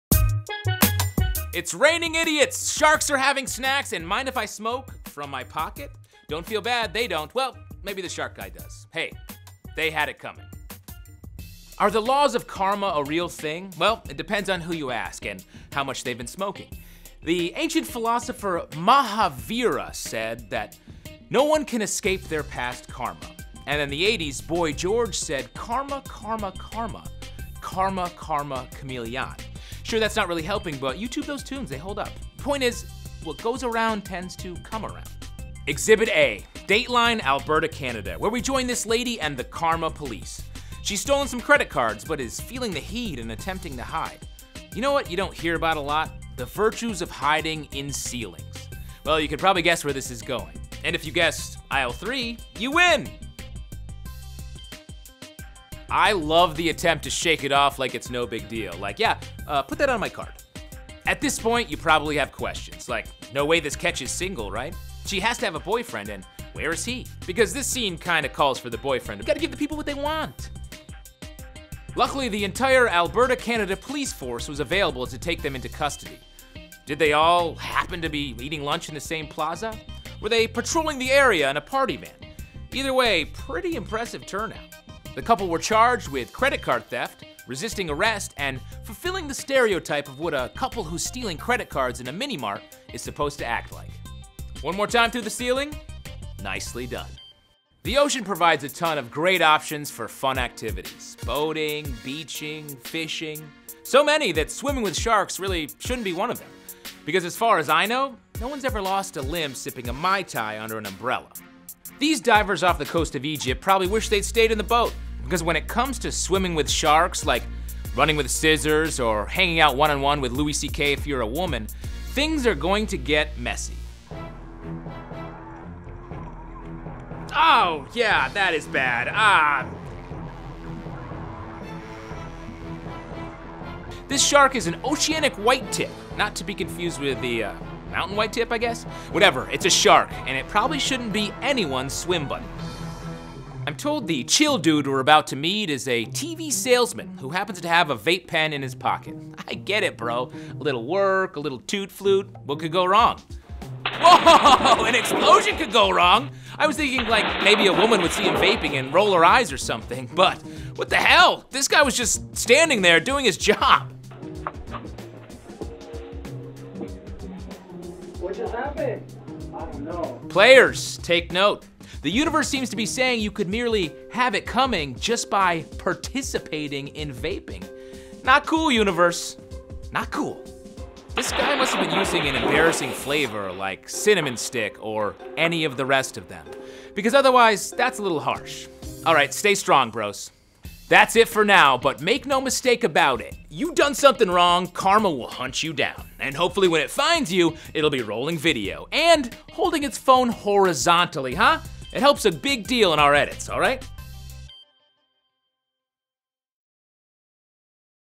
Instant Karma Compilation: Shark Bites Diver, Robber Thru Roof, E-Cig Explosion | They Had It Coming
In this episode of "They Had It Coming," we learn why you shouldn’t go swimming with sharks. Plus, a would-be convenience store robber ends up falling through the ceiling, and an e-cig explodes in this poor guy's pocket.

Clip 1
Shark attacks diver in Egypt
A group of divers were shocked when an oceanic whitetip shark latched onto a diver’s leg in the waters off the Brother Islands in Egypt. When the shark eventually let go, the victim was taken back to the boat and rushed to a nearby hospital, where he was treated for his injuries.

Clip 2
Dimwit falls through the ceiling while hiding from police
A pair of alleged credit card thieves were apprehended in a convenience store in Alberta, Canada. When approached by the police, the female member of the duo tried to evade arrest by hiding above the store’s ceiling panels, only to come crashing through to the floor.

Clip 3
E-cig blows up TV salesman's crotch
A worker at an HDTV Outlet in Anaheim, California, suffered from burns when an E-cigarette in his pocket exploded. He suffered from burns, although he was able to drive himself home after the incident.